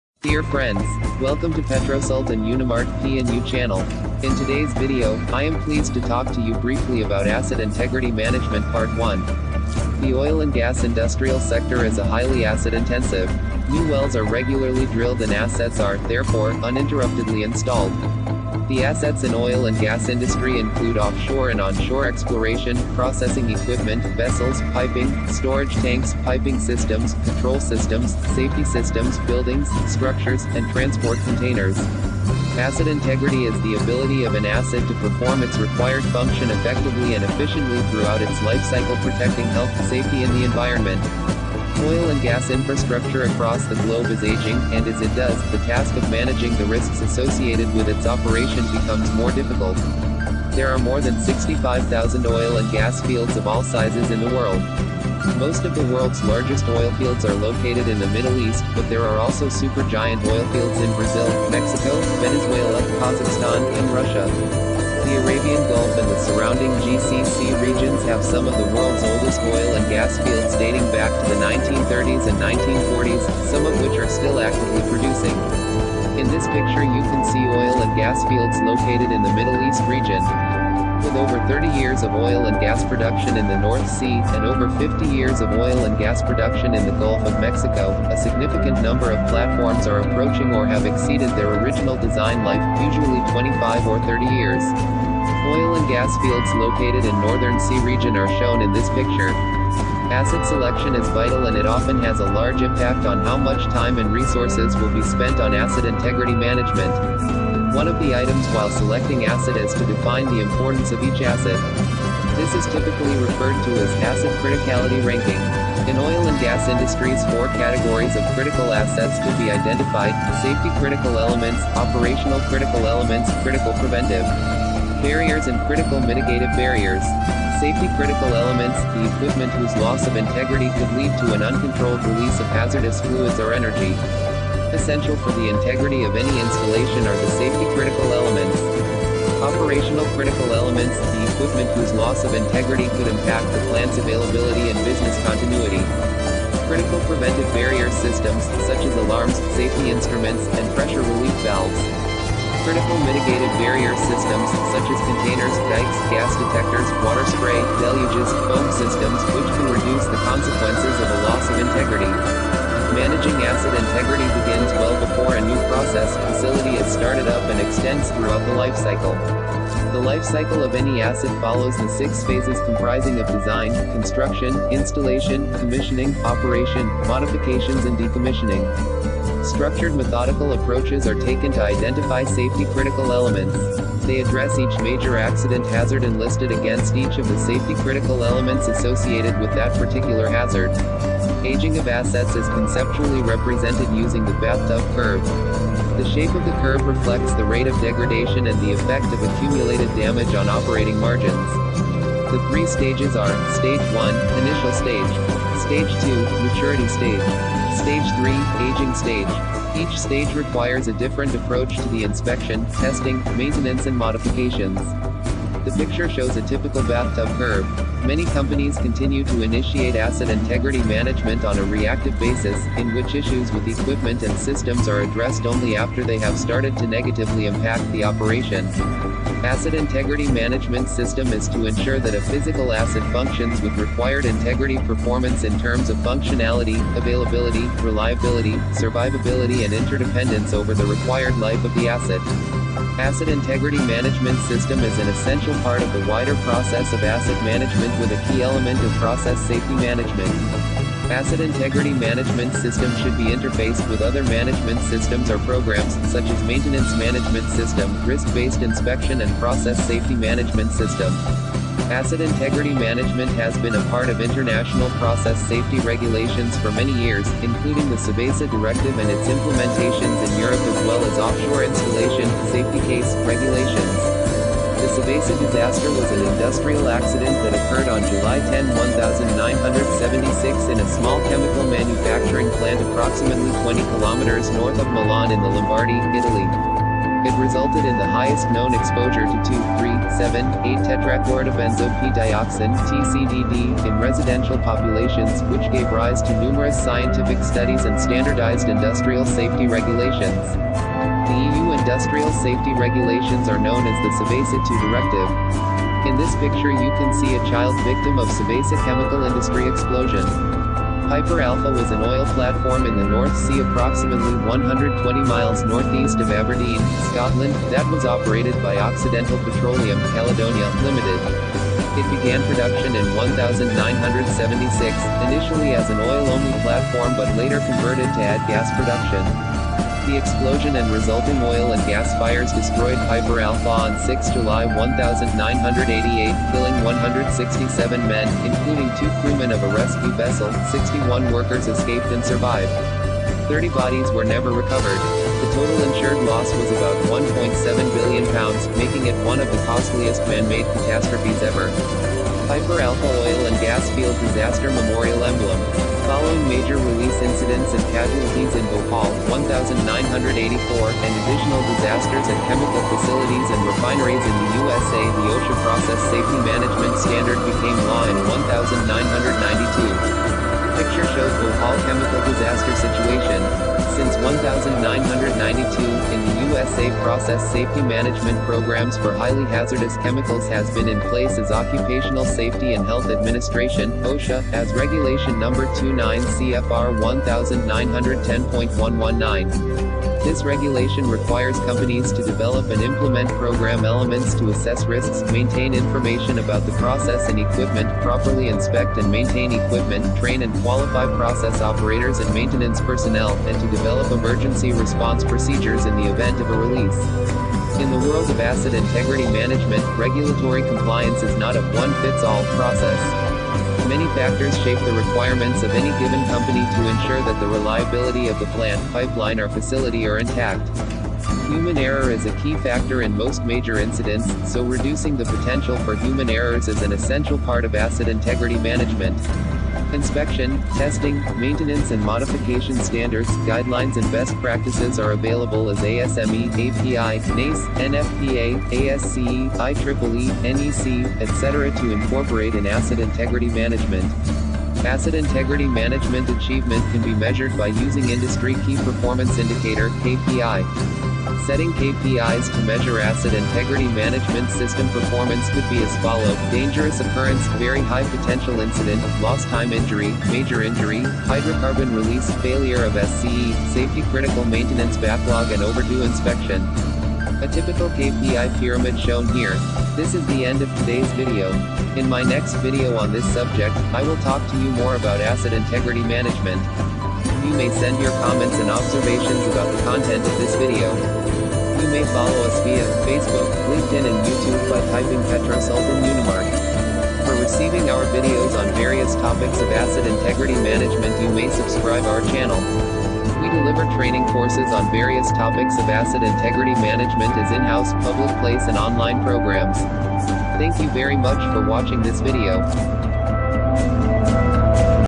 ASSET INTEGRITY MANAGEMENT
Asset integrity management system is to ensure that a physical asset functions with required integrity performance in terms of functionality, availability, reliability, survivability and interdependence over the required life of the asset.